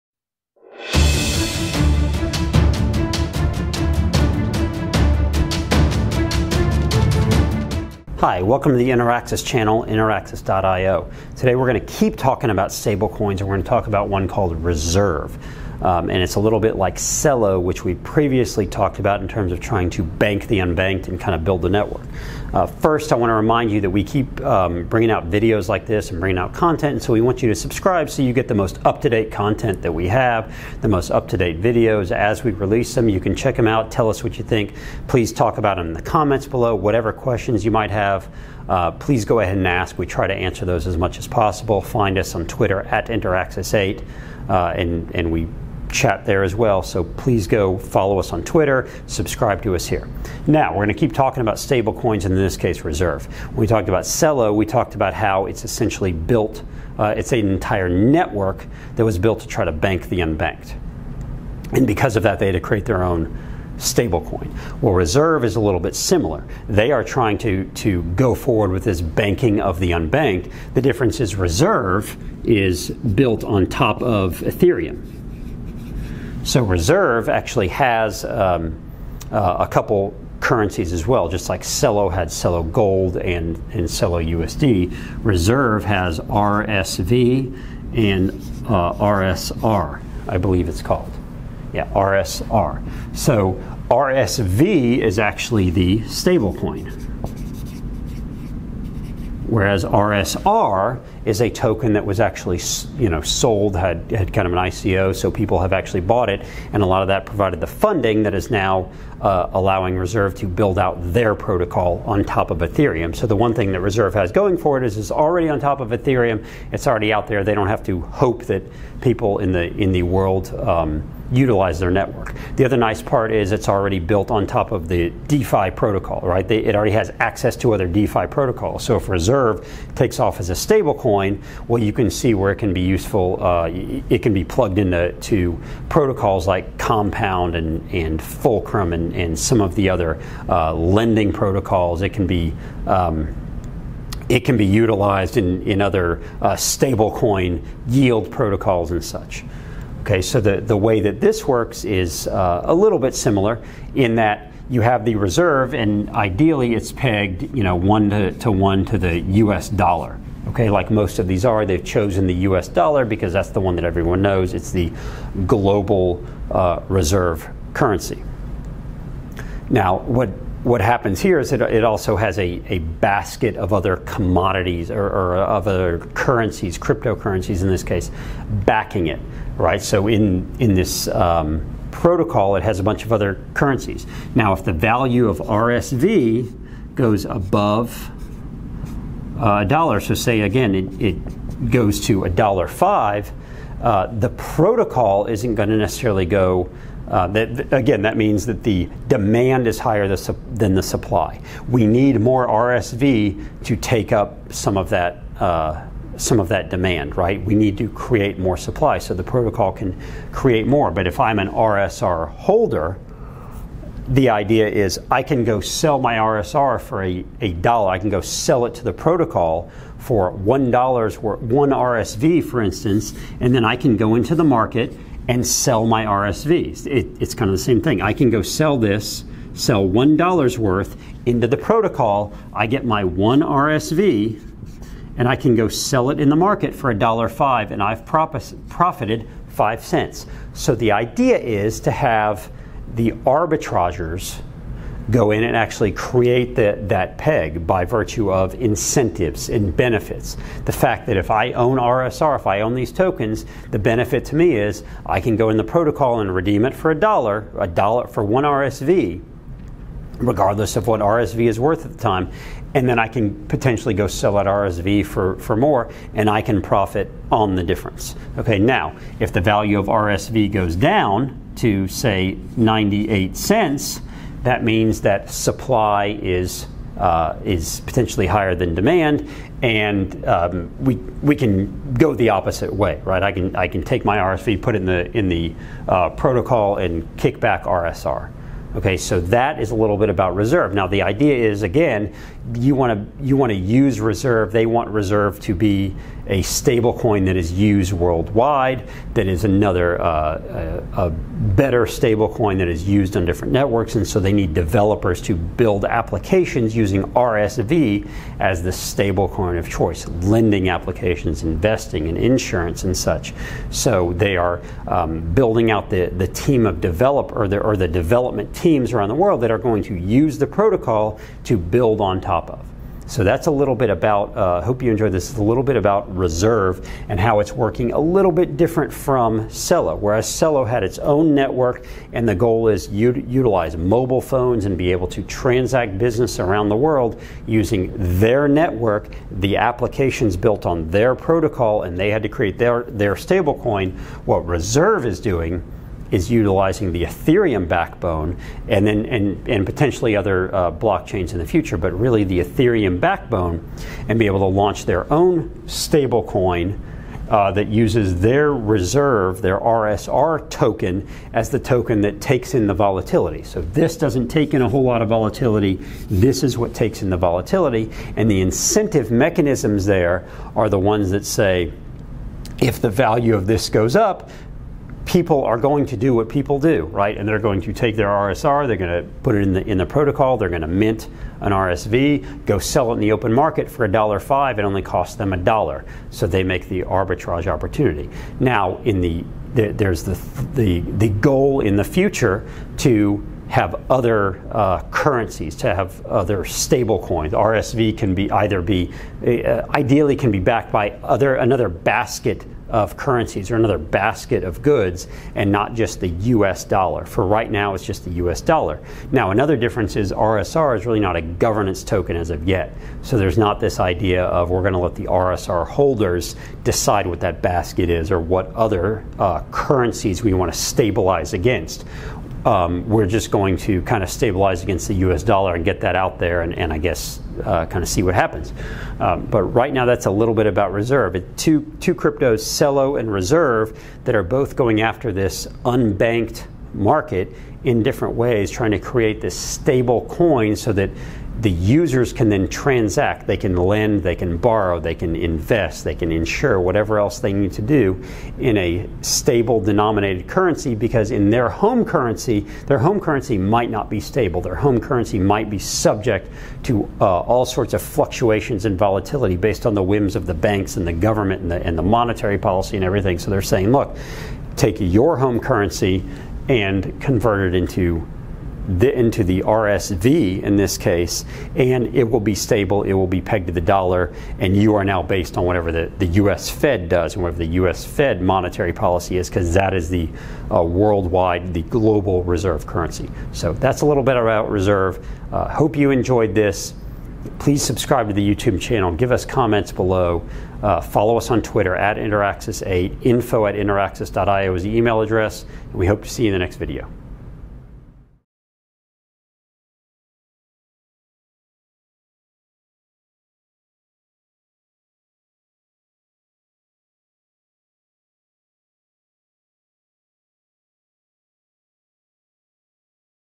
Reserve - A Stablecoin to Bank the Unbanked | Interaxis.io
In this video we discuss another Stablecoin that is looking to help bank the unbanked.

Reserve has the RSV token, which is the Stablecoin, and RSR, which the token that fluctuates in value.

The tokens are built on the Ethereum Blockchain, and are made to maintain a 1:1 peg to the US dollar currently.  Eventually the team plans to peg the value to a basket of currencies or goods, to maintain more worldwide stability.

The interplay of RSR and RSV is such that users and investors are counted on to perform the arbitrage necessary to maintain the peg.  The users are incented to buy or sell RSV for RSR when the price deviates from $1.

The idea is to build applications that use the RSV stablecoin as the means of transaction for payments, but also for loans, investments, etc., with the goal of creating a peer-to-peer, International network.